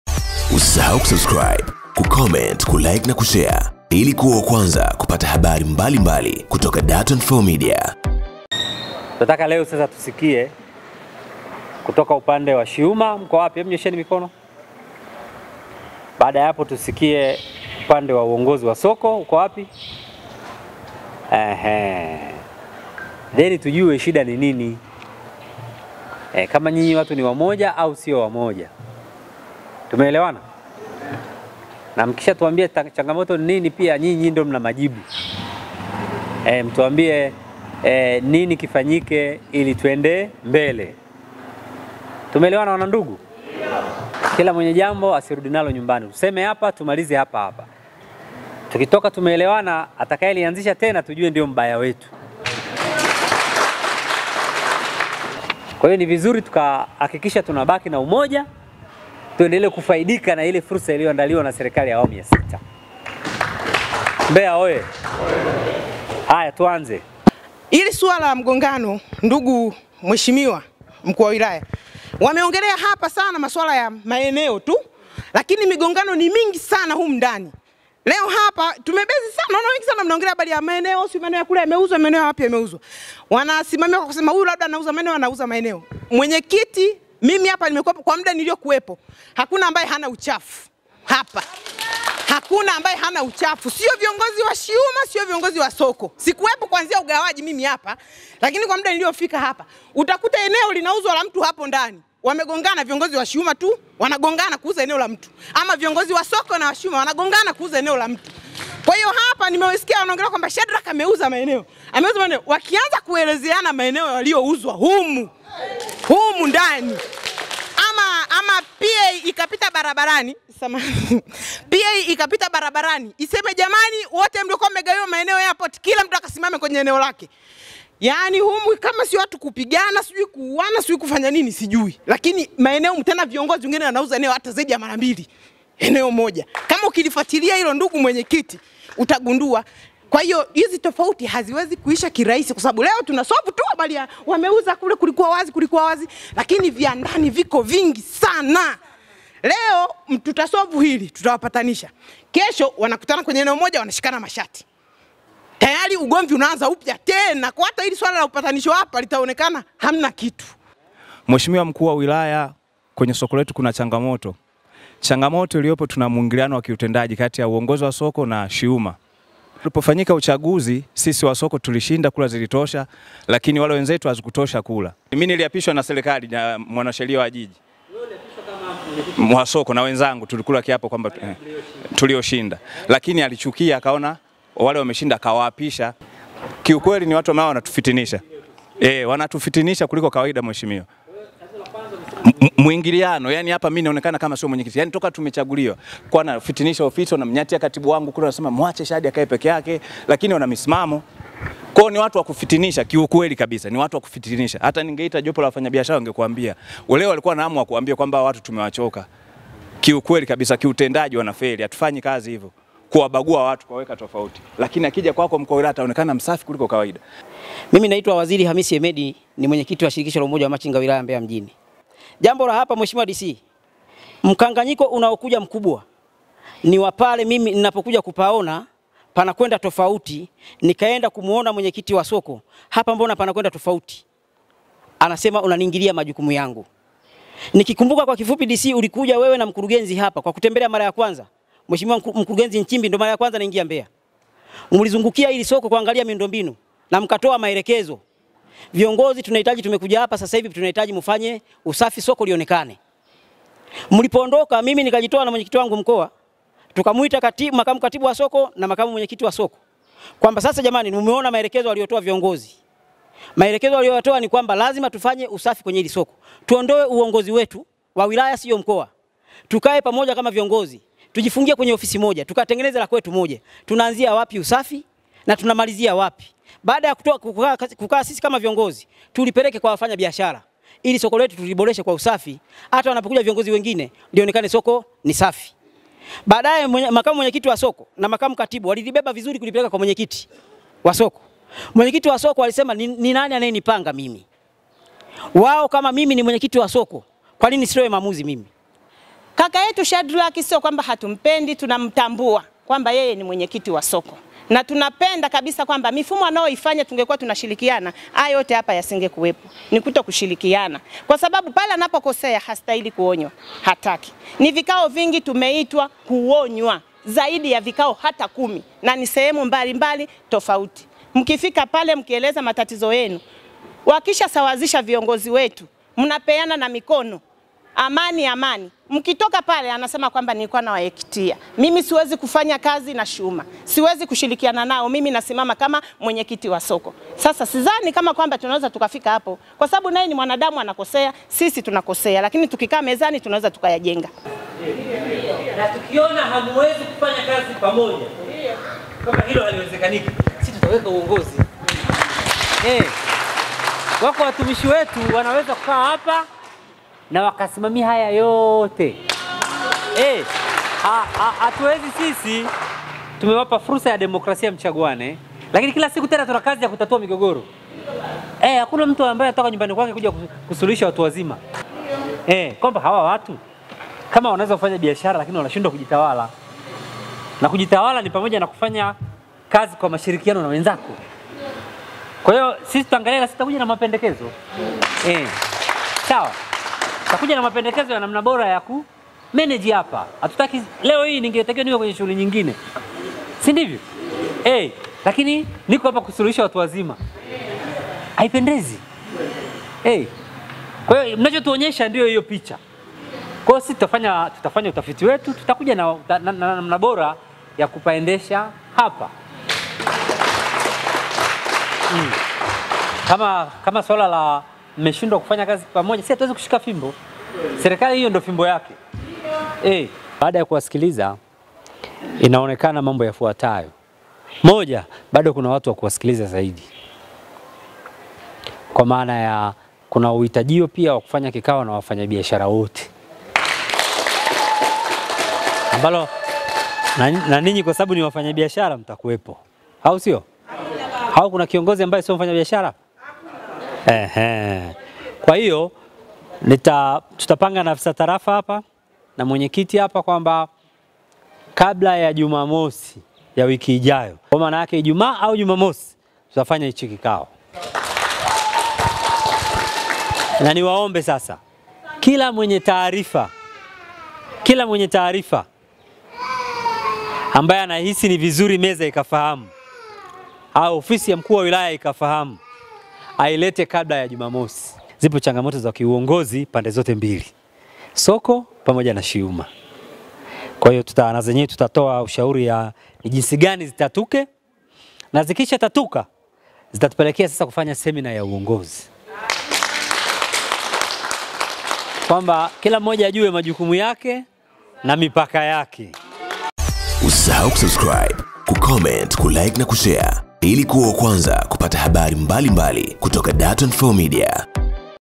DC MALISA AINGILIA KATI MGOGORO SOKO LA WAMACHINGA / TUMIENI FURSA/ KUZISIKIKIZA PANDE ZOTE
Serikali imeingilia kati mgogoro uliopo kati ya Viongozi wa Soko la Machinga la Old Airport na Viongozi wa Shirikisho la Umoja wa machinga - SHIUMA, Jijini Mbeya uliokuwa ukiathiri ukuaji wa Soko na kudhoofisha maendeleo ya Wafanyabiashara Sokoni hapo.